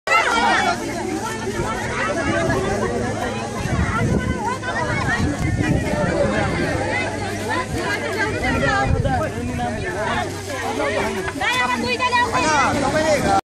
Nepal Earthquake 2015 damage School in Gorkha
This is in Ghalyang, Gorkha District ,Nepal...this school is destroyed very badly by the devastating Earthquake